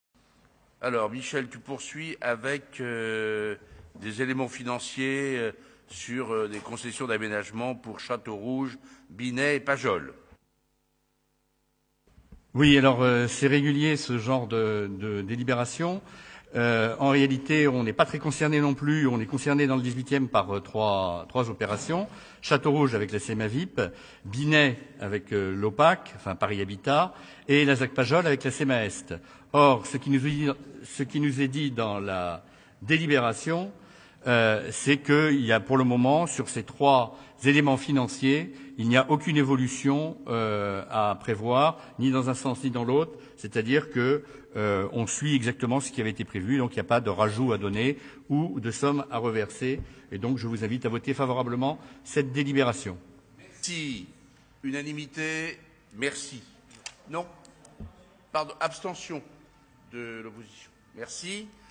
08/12/08 - URBANISME
Opérations Château rouge, Binet et Zac Pajol, /
Approbation des éléments financiers des traités de concession d’aménagement pour les opérations Château rouge, Binet et de la Zac Pajol, arrêtés au 31 décembre 2007